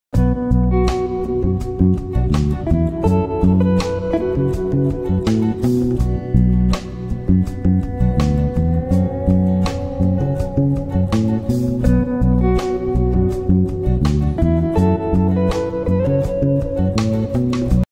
Paint with me a whimsical berry branch#gouachepainting
Paint with me a whimsical berry branch!

It’s tough for me to stick to one brush so I pick whatever feels right. Here I’ve used Princeton Neptune 4 and Royal & Langnickel  0